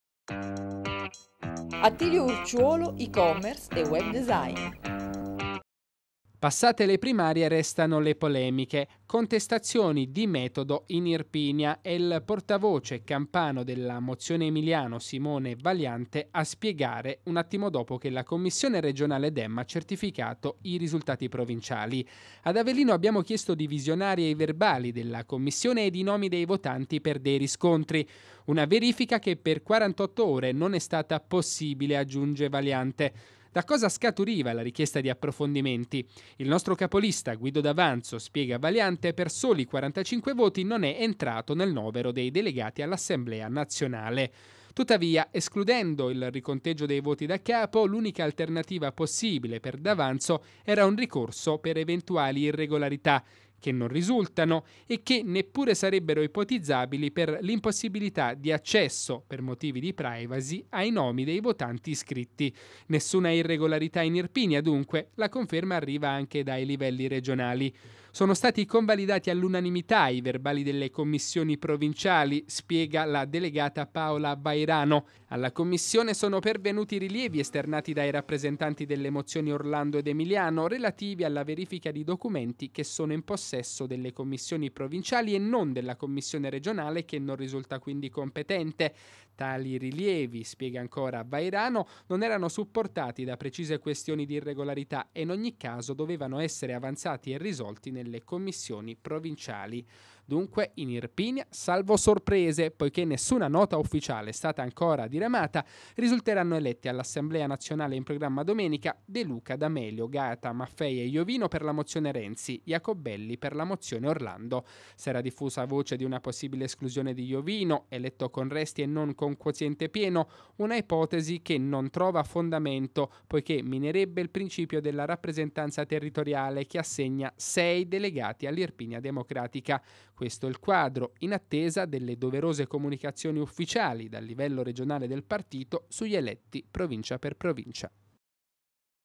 Primarie PD, D'Avanzo sfiora l'elezione tra le polemiche
D'Avanzo Per soli 45 voti fuori dall'assemblea nazionale PD. Il portavoce regionale di Emiliano:”ad Avellino verifica possibile solo dopo 48 ore “